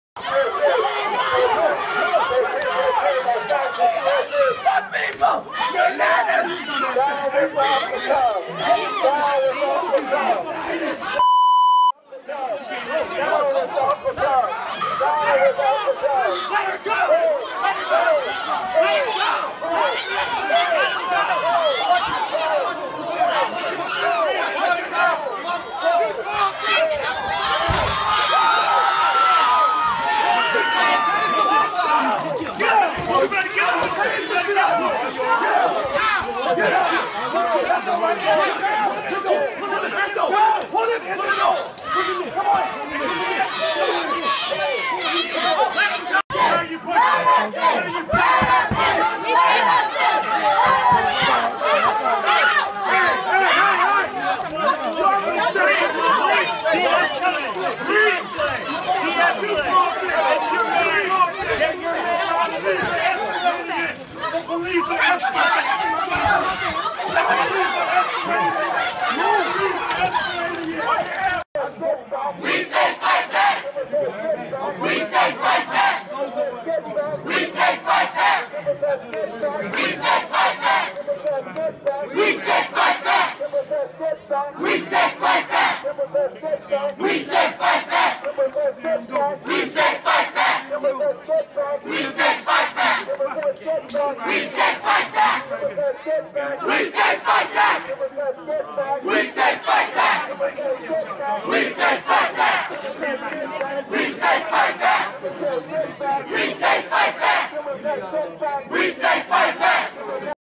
Protesters Scuffle With Police in Attempt to Barge Into Louisiana Senate Debate Venue
Courtesy of Twitter/nicksreimann